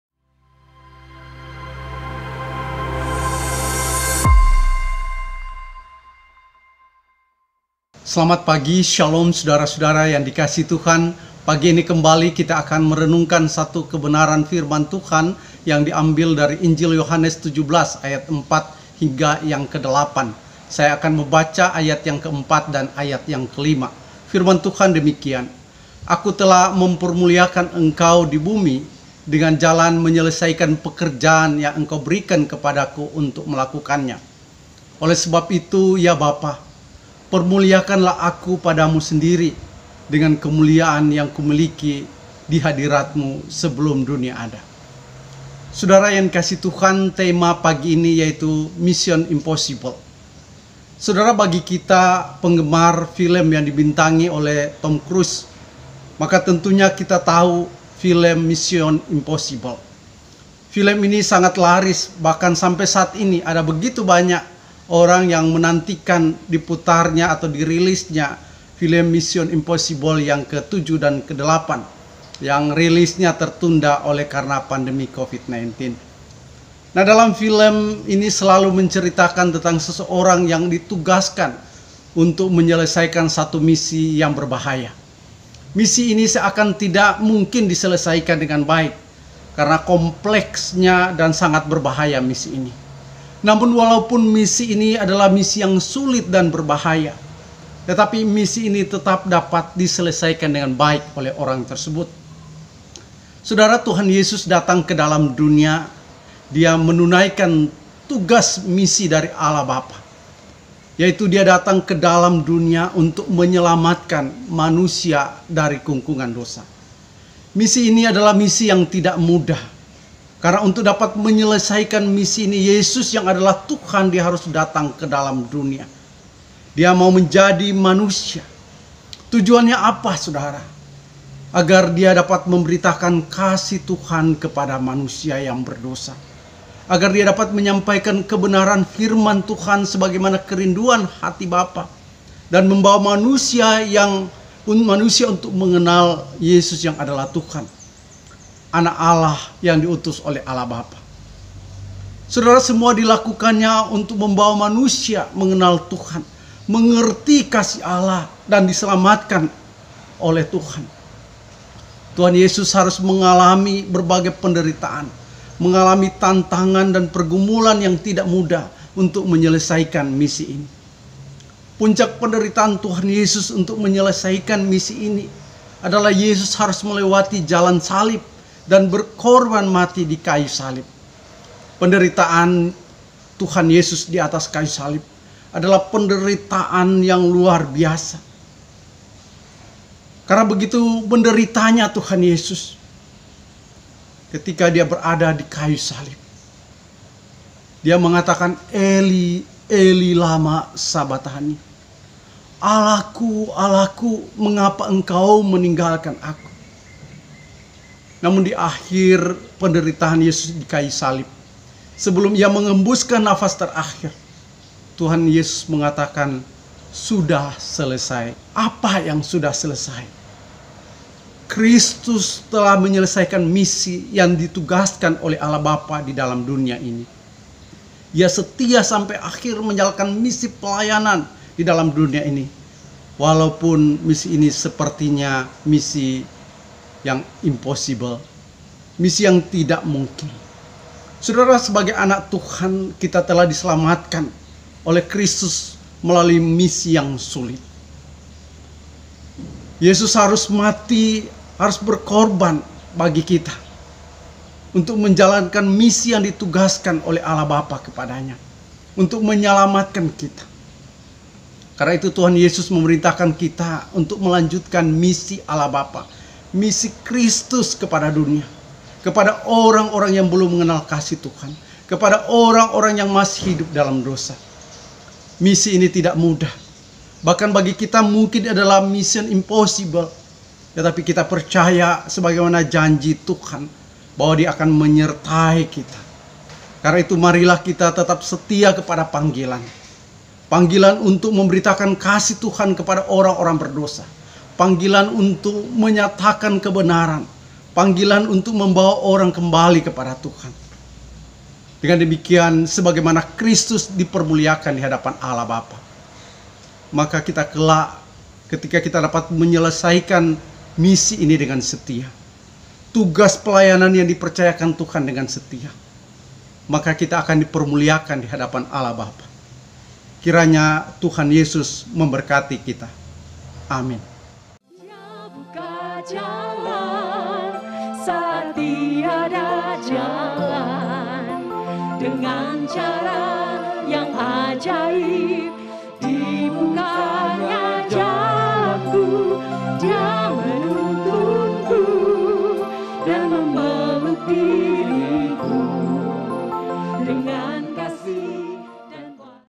Pdt. Beni S. Regoh - Mission impossible
Renungan Harian - GKKB Jemaat Pontianak
Seri Injil Yohanes: Mission impossible (Yohanes 17:4-8)